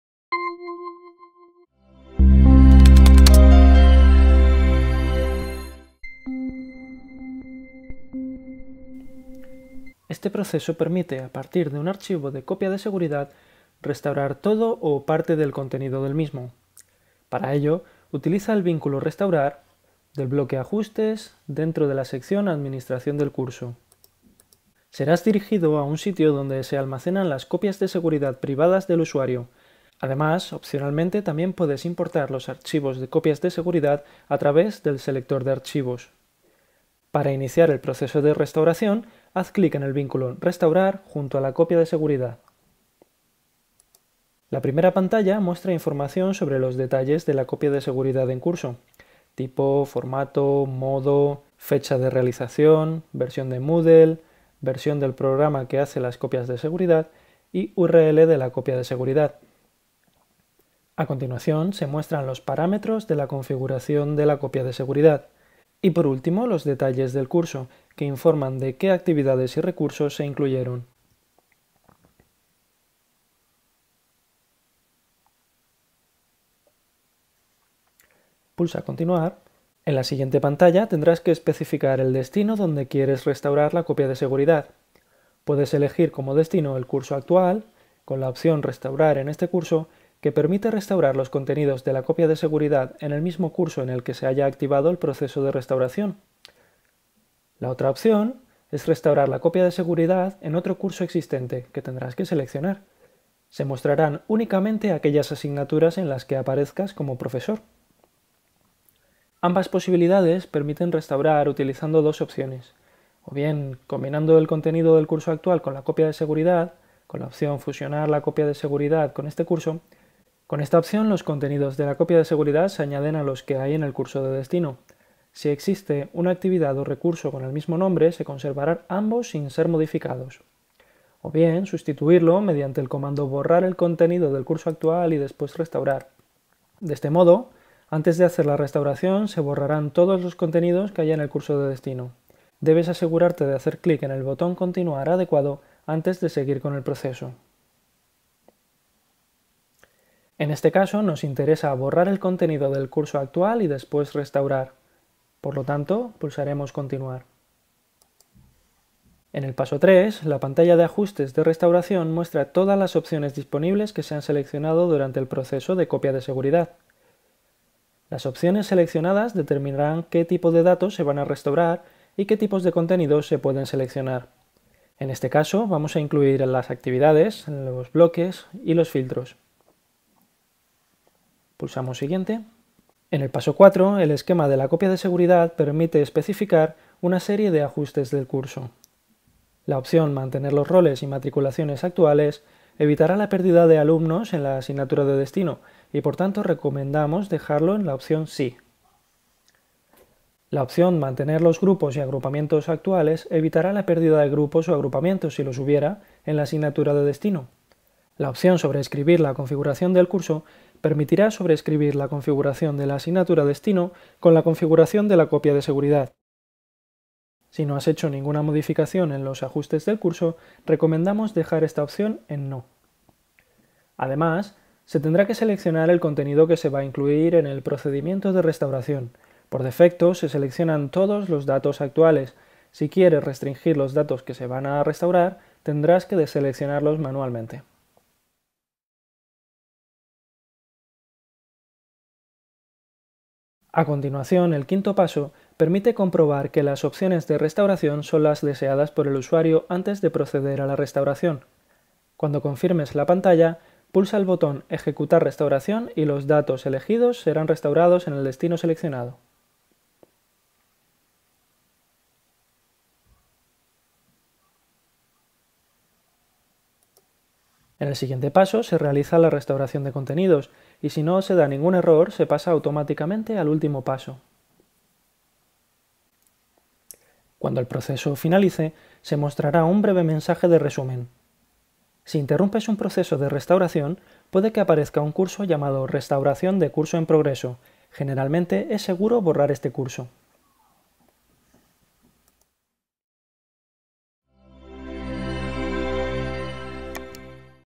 Restaurar una copia de seguridad (Moodle 2.6)
Vídeo explicativo sobre cómo restaurar una copia de seguridad en una asignatura, dentro de la plataforma Moodle.

Curso Avanzado de Moodle 2.6 URJC